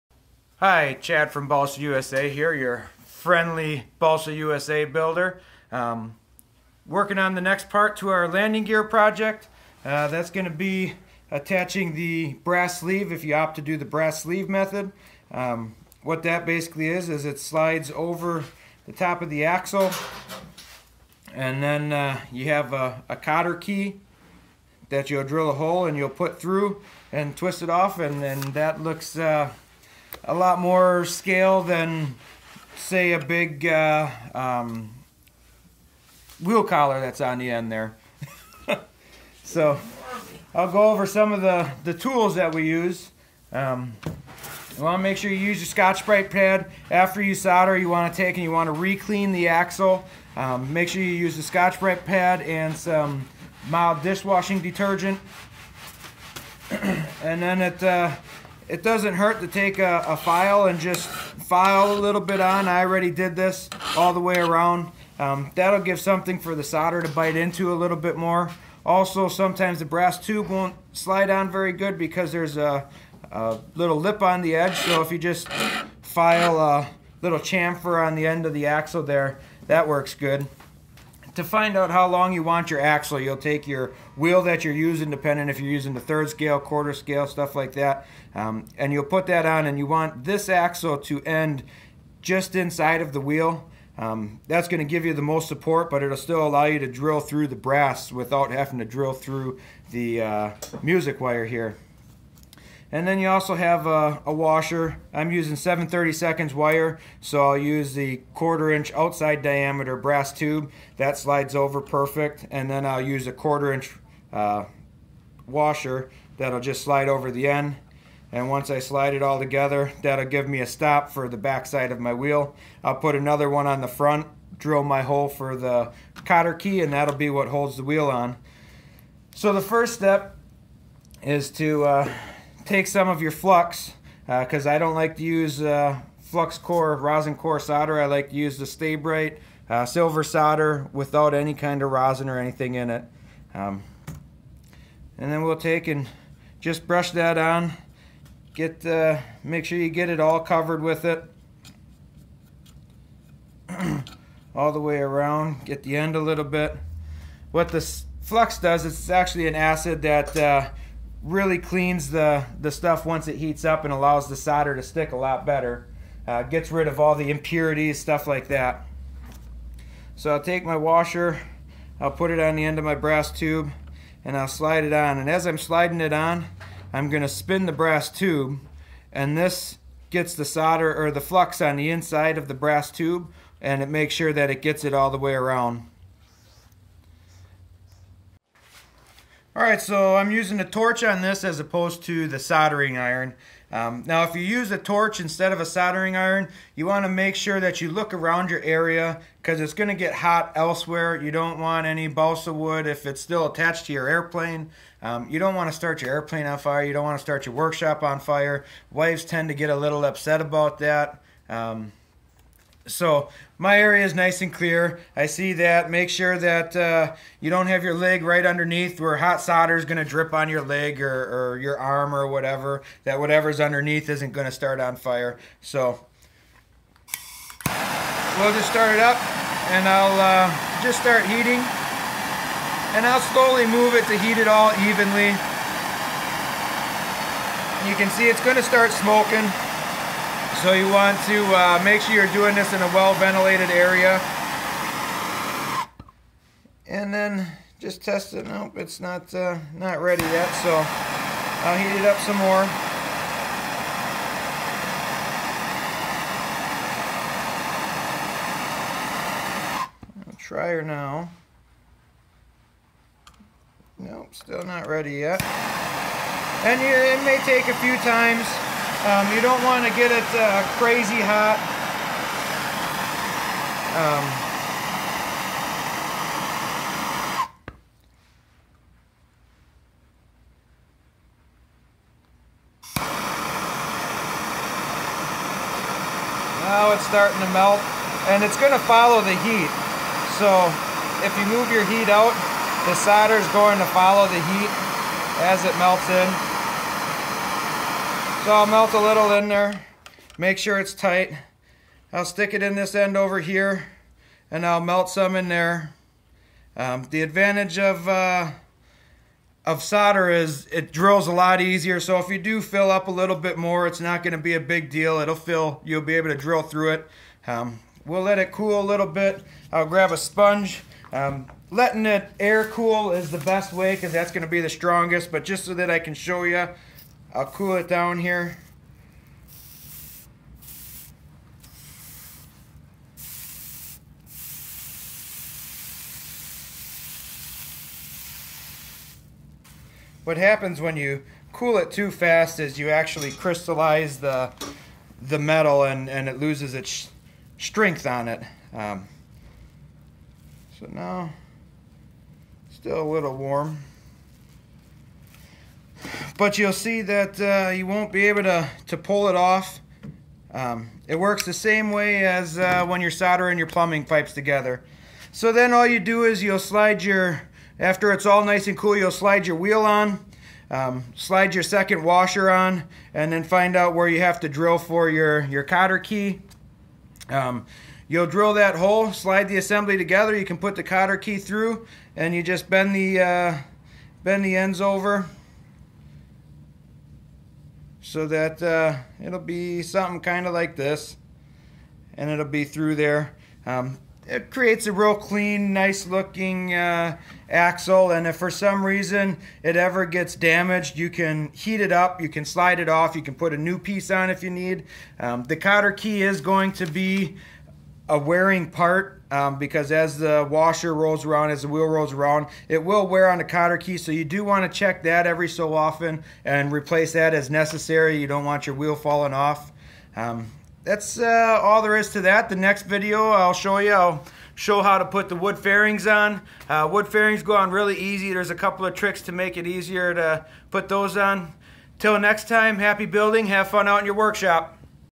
Balsa USA Brass Axle Installation Tutorial
Installing a Brass Axle on your existing Landing Gear